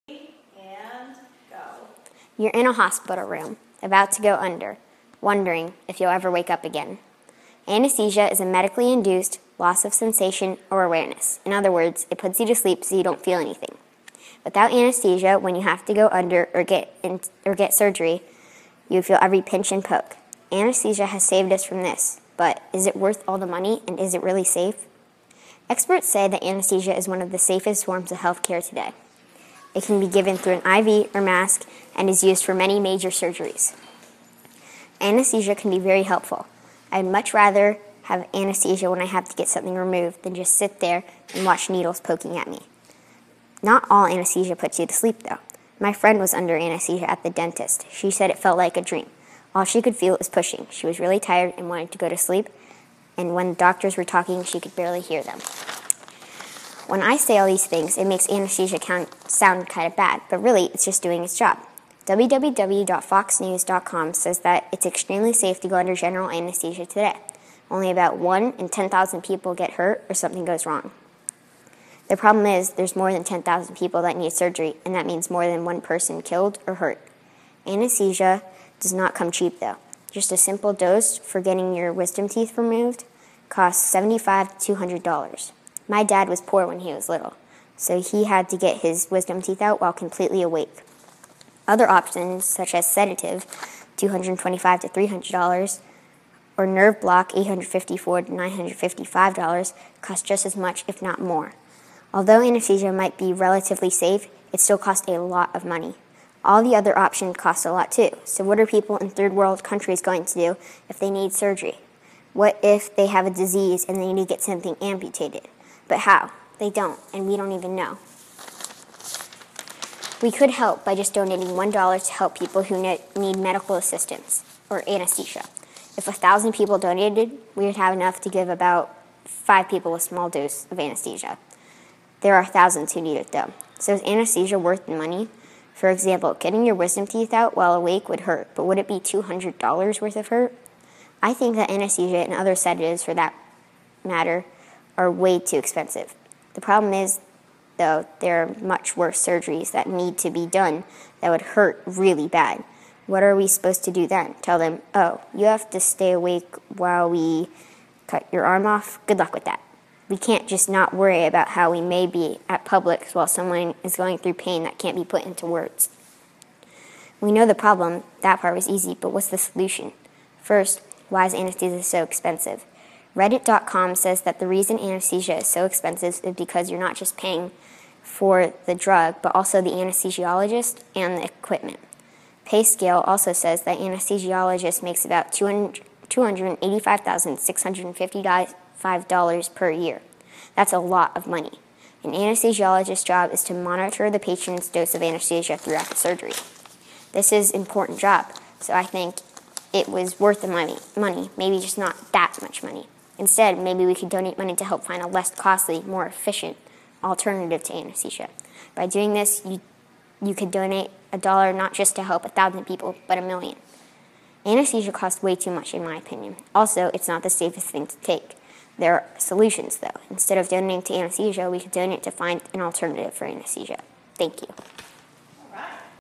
Why does anesthesia cost so much money and is it safe? | Emily Kneisly | Roosevelt Elementary School
Anesthesia is really a scary thing, but so necessary to avoid pain and provide safety during medical procedures.  Necessary and actually really safe - but expensive!  Why does it cost so much and is that fair?

Emily is a graduated gifted student from Roosevelt Elementary in Tampa.